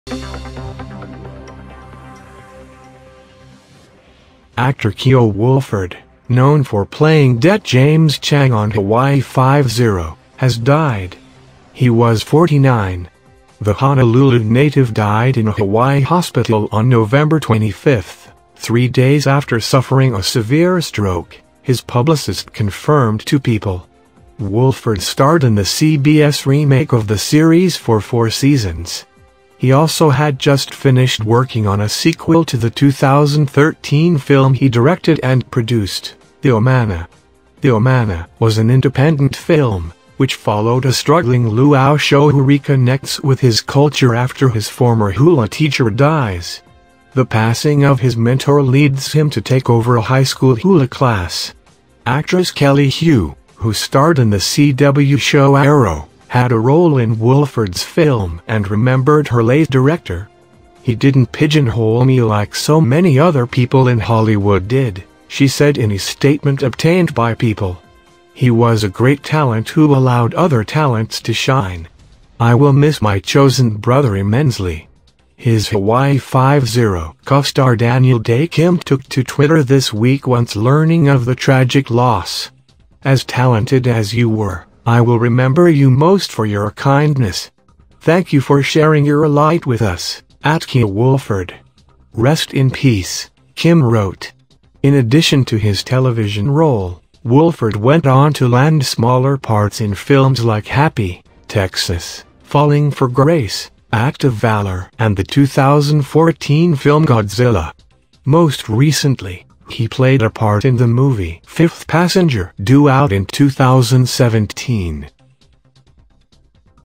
Keo Woolford, ‘Hawaii Five-0’ actor, dead at 49
Actor Keo Woolford, known for playing Det. James Chang on “Hawaii Five-0,” has died.